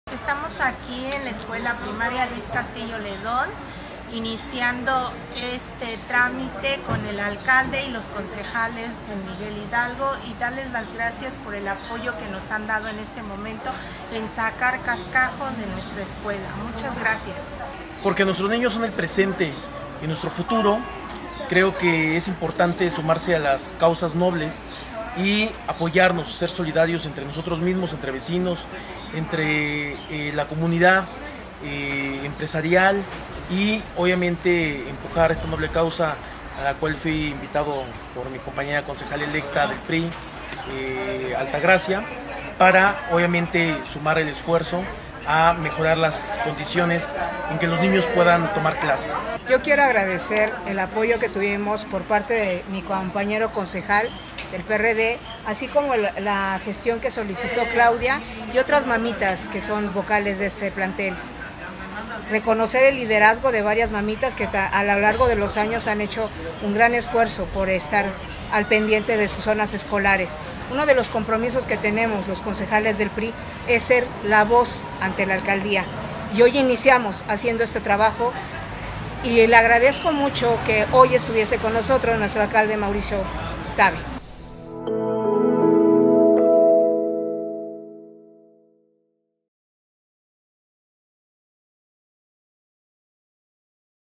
Los Concejales de MH se unen para atrás un regresoaclases seguro
Gracias a los concejales Oscar Munguía y Altagracia Méndez por sumarse a un RegresoAClases seguro para las y los pequeños de Miguel Hidalgo.